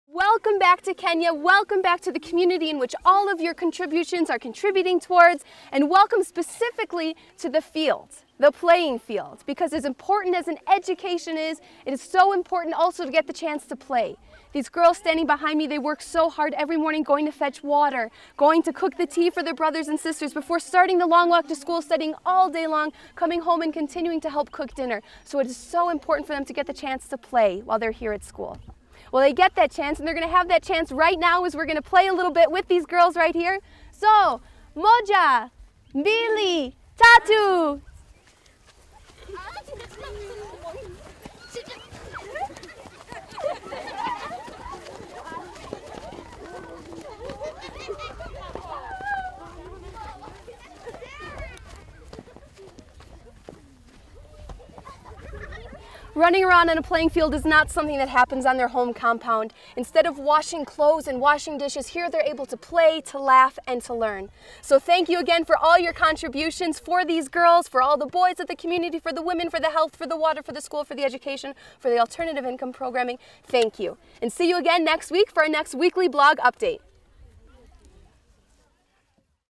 THE Onederworld in Kenya: Welcome to the Playing Field!
10 Aug 2009: Everyday life for girls in the Pimbiniet community is filled with chores. Apart from doing the washing and helping with the preparation of the evening meal, they have to wake up at dawn to go and fetch water from the river, 6km away. After carrying the heavy containers back home, they then have to make tea for the family before starting the long walk to school. That's why it is so important that they get a chance to play during their school day!

We at THE Onederworld recognise that playful physical activity is a significant part of holistic education. It not only improves the health of the girls but encourages them to try new things and take on new roles within their community. That's why teachers are encouraging girls to get active by providing opportunities for them to participate in school sports. Sport is a great way to challenge traditional gender norms, improve the confidence of girls and provide fun activities that ensure everyone enjoys their time at school.